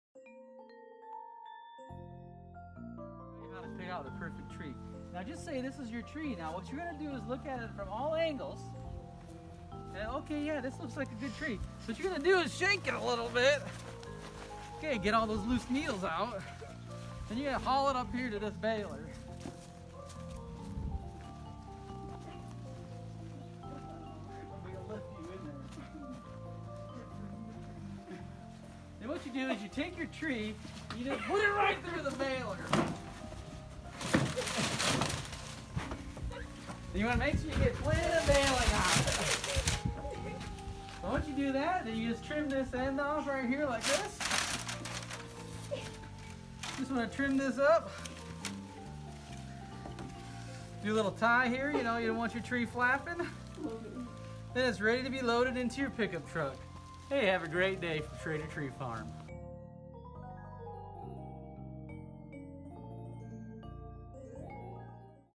How to select the perfect Christmas tree
How to select the perfect Christmas Tree. The Schrader Tree farm style!